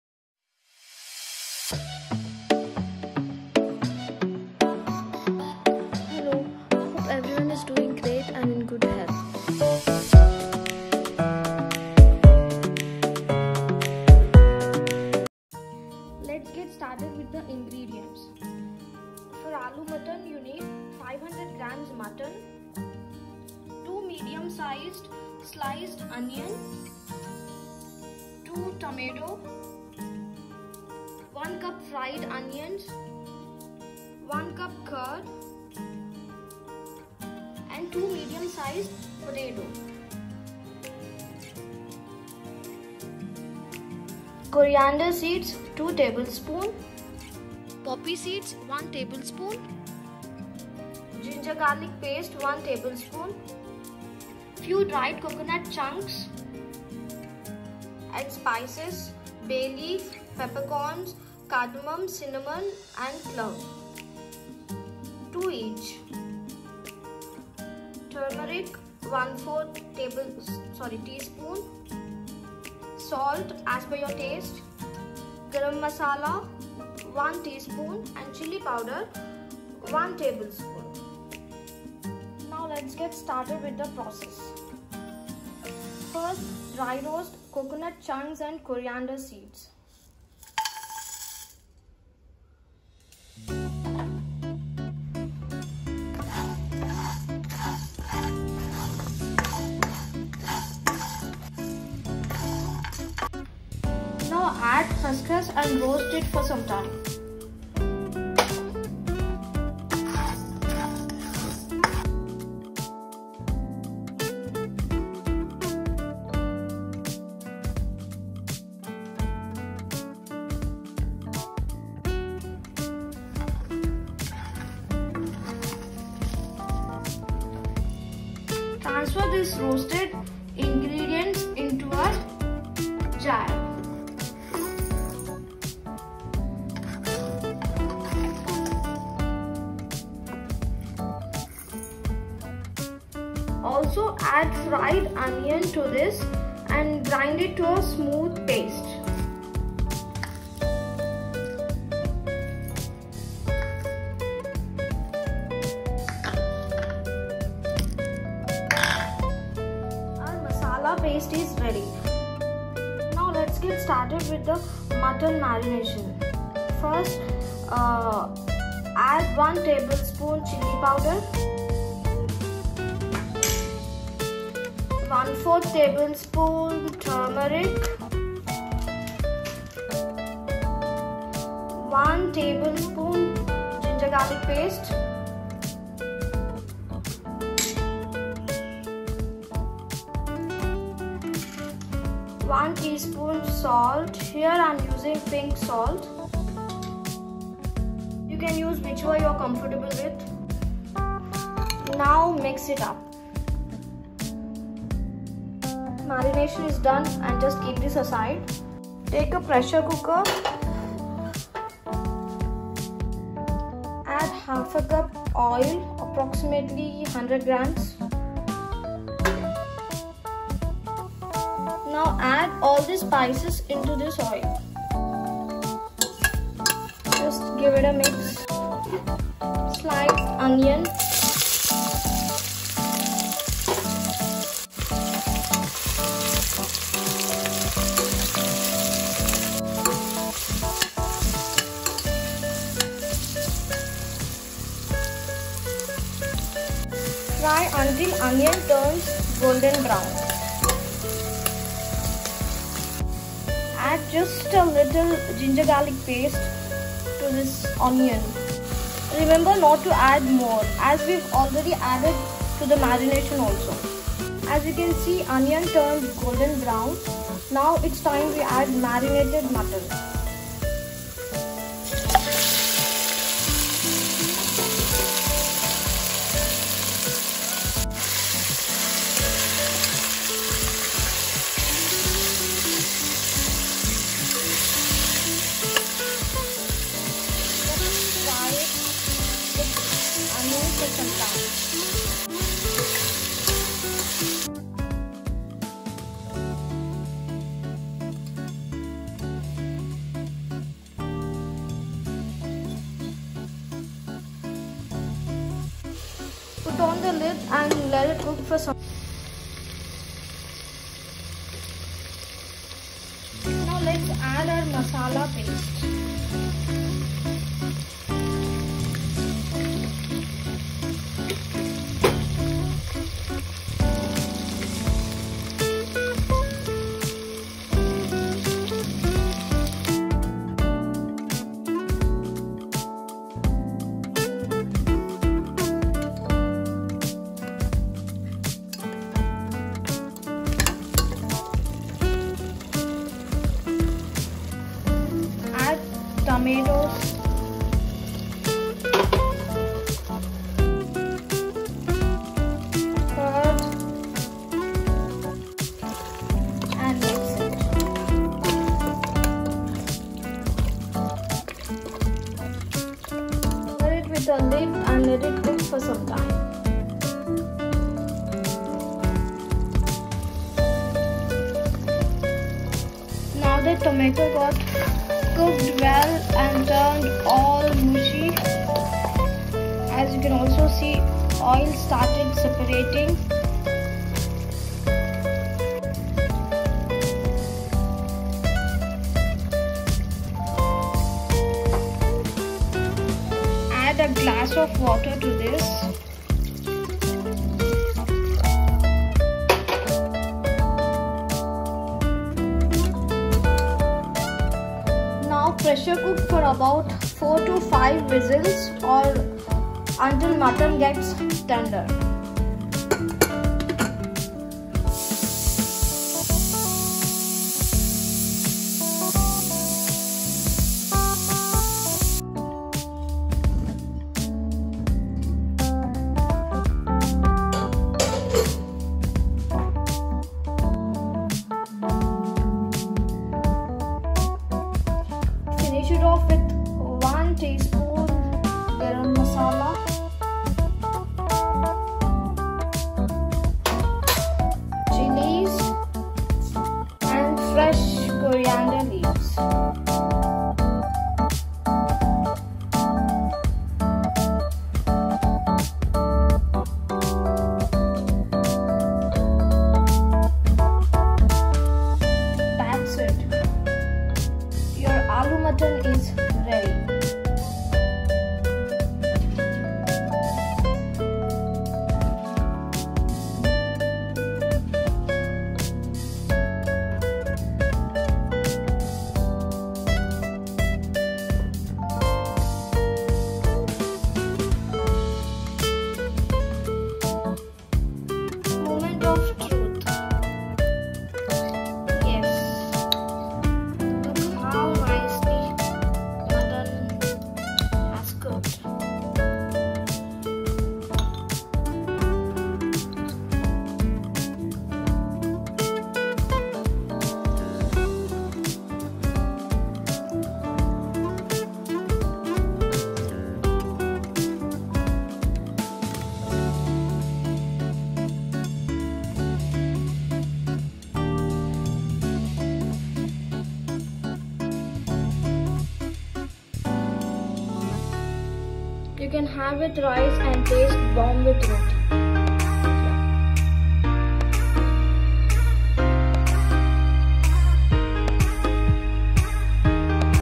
ALOO MUTTON CURRY|ALOO GOSHT|MUTTON RECIPE|BEST POTATO MUTTON
💥Aloo Mutton💥
🔷INGREDIENTS
▪️Mutton 500grams
▪️Onion 2medium sized
▪️Tomato 2 no
▪️Fried Onion 1cup
▪️Curd 1cup
▪️Potato 2medium sized
▪️Coriander seeds 2tbsp
▪️poppy seeds 1tbsp
▪️Ginger Garlic paste 1tbsp
▪️Dry Coconut chunks few
▪️Bay leaf,Cardamom,Cinnamon,Clove 2each
♦️METHOD
🔹Dry roast coconut chunks,coriander,poppy seeds for 2 minutes.
🔹Transfer roasted ingredients to a jar also add fried onion and grind it to a smooth paste.
🔸Mutton Marination
🔺Add 1tbsp chilli powder,1/4 tsp turmeric,1tsp ginger garlic paste,1tsp salt to the mutton mix well and keep aside.
🔺Take a pressure cooker add 1/2cup oil and whole spices(bay leaf,cardamom,cinnamon,clove)turns aromatic.
🔺Now add sliced onion and fry till
Onion becomes golden brown.
🔺add little GG paste(optional)
🔺Add marinated mutton and fry mutton with onion for 5 minutes.
🔺Put on the lid and cook for 5 minutes on low medium flame.
🔺Now add masala paste prepared earlier and mix well.
🔺Add chopped tomatoes,curd and potatoes and mix it all.
🔺Cover and cook for 10 minutes or till tomatoes become soft and oil separates.
🔺Now add a glass of water and pressure cook for 5 vissels or till mutton gets tender.
🔺Finish it off by adding 1tsp garam masala,green chilli’s and chopped coriander.
🔹That is itttt!your aloo mutton is Ready💥
Have with rice or tastes amazing with roti!